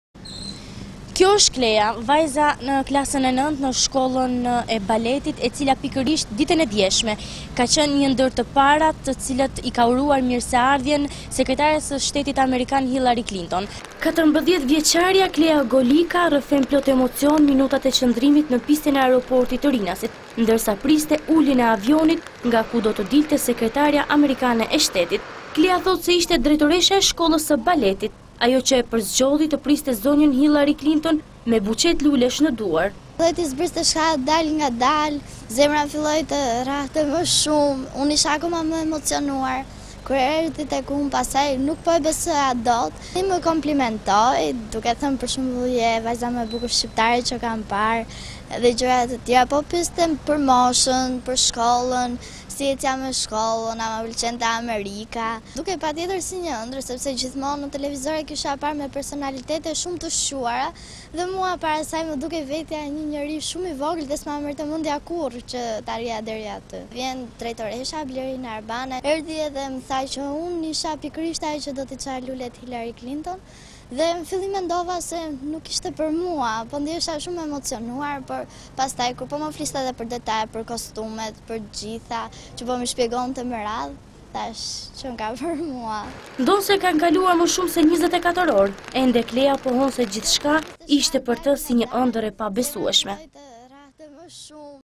Intervista/Vajza e luleve: Hillary më tha do jesh e famshme
Klea Golikja, 14-vjeçarja nga Tirana, është personi i parë që takoi sekretaren amerikane të shtetit, Hillary Clinton, sapo ajo zbriti dje në aeroportin e Rinasit. Nxënëse në klasën e nëntë te shkolla e Baletit, në një intervistë për "Shqiptarja.com", Klea rrëfen emocionet e këtij takimi të pazakontë për të. Ajo shprehet se mbeti e befasuar nga thjeshtësia e sekretares së shtetit, e cila gjatë momentit kur i ka dhuruar lulet, jo vetëm nuk i ka kursyer komplimentet për të, por madje i ka shprehur dëshirën për ta takuar sërish në të ardhmen.